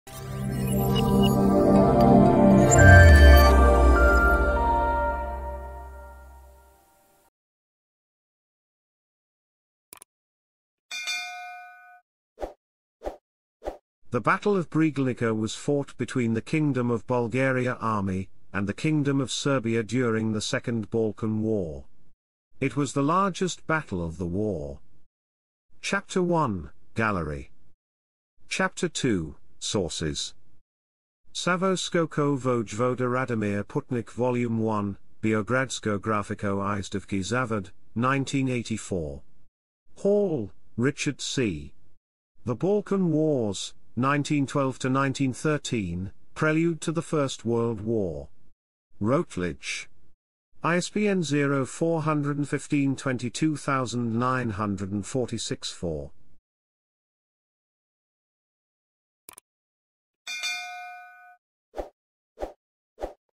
Battle of Bregalnica - Wikipedia article
Video summary:
The Battle of Bregalnica was fought between the Kingdom of Bulgaria army and the Kingdom of Serbia during the Second Balkan War. It was the largest battle of the war.